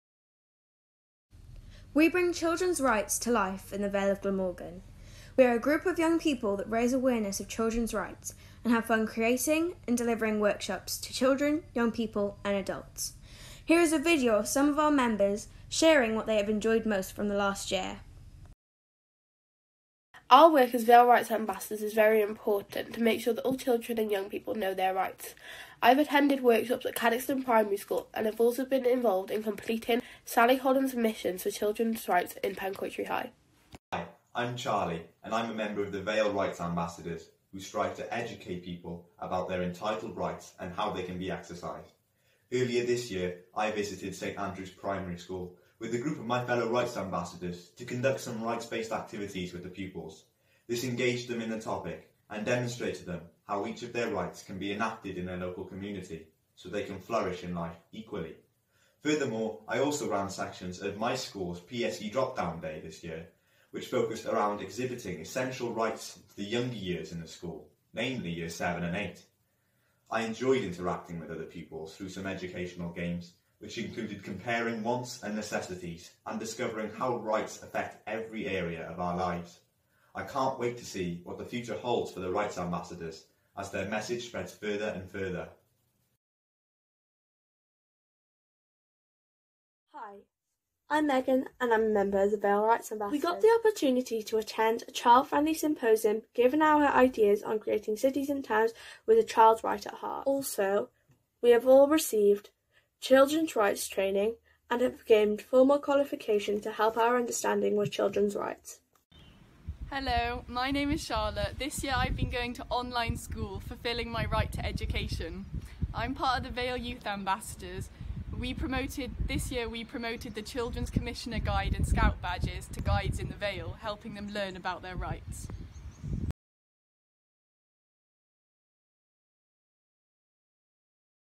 Vale Rights Ambassadors Project Video 2021
Rights Ambassadors talking about what they have enjoyed the most from the being a member of the project.